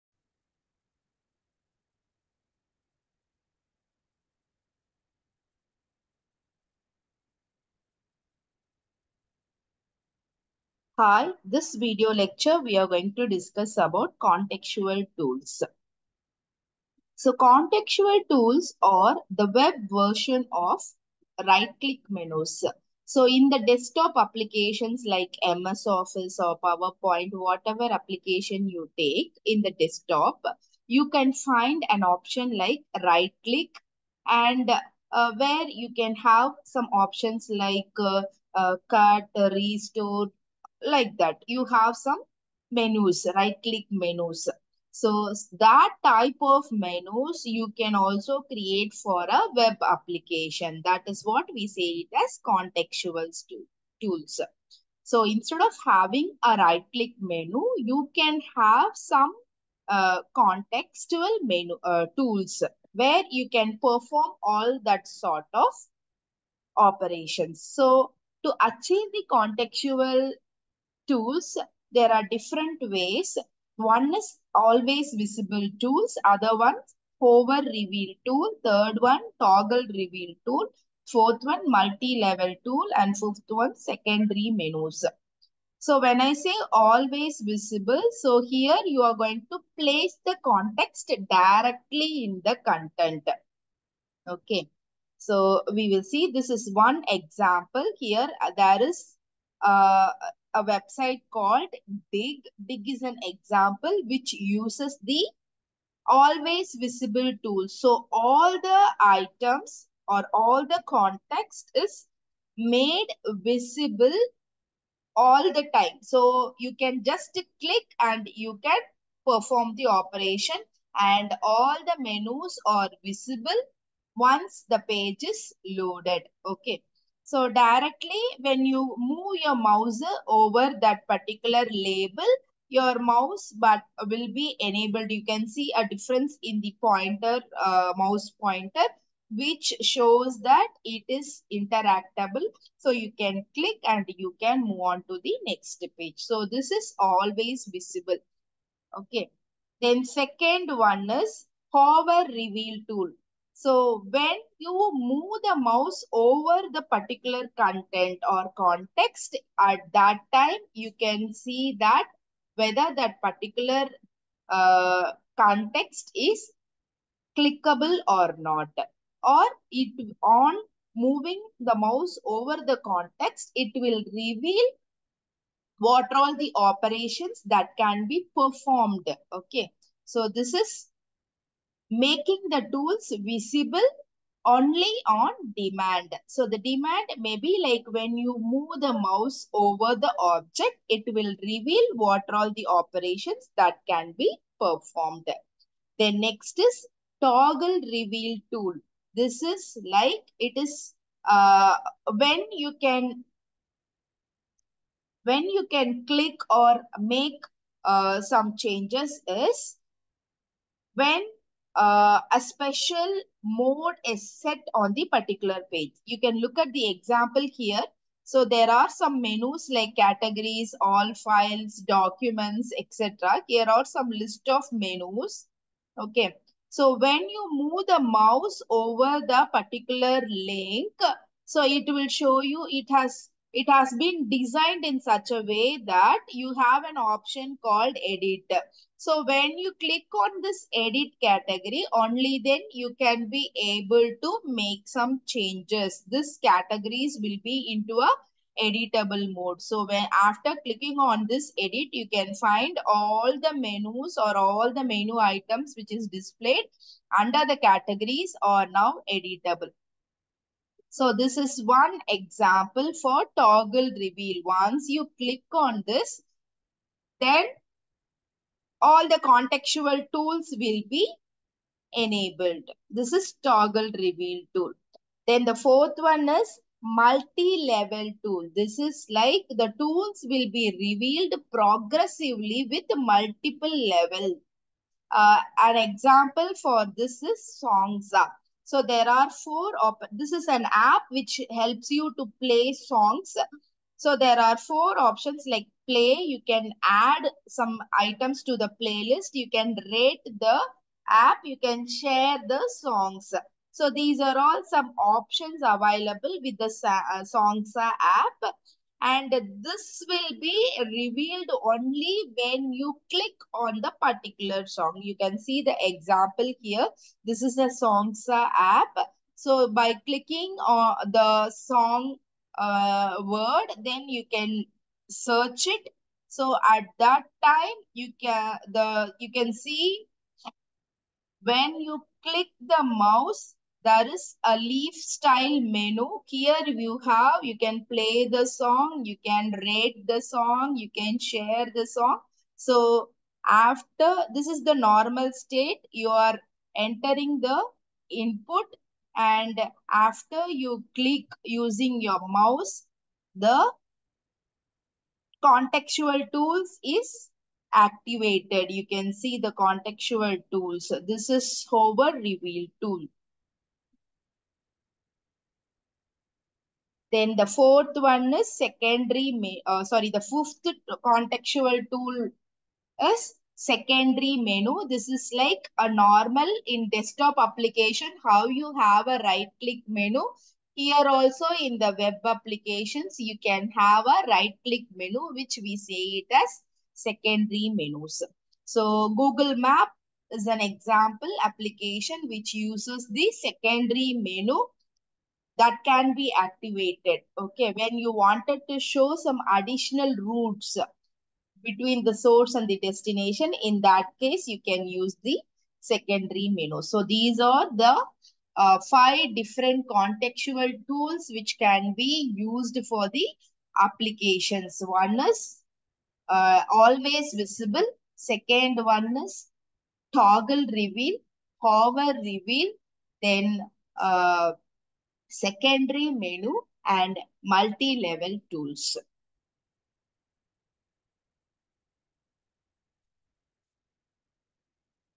Contextual Tools | Human Computer Interaction, Mrs. A. Tamizharasi, AP/CSE, RMDEC
This video explains on the Contextual tools